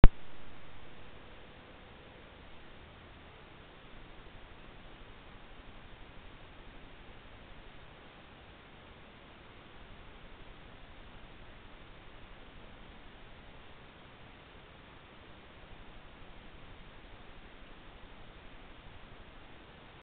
Ginsterkatze in Namibia
Eine Kleinflecken Ginsterkatze am Bait auf der Jagdfarm Moselle in Namibia.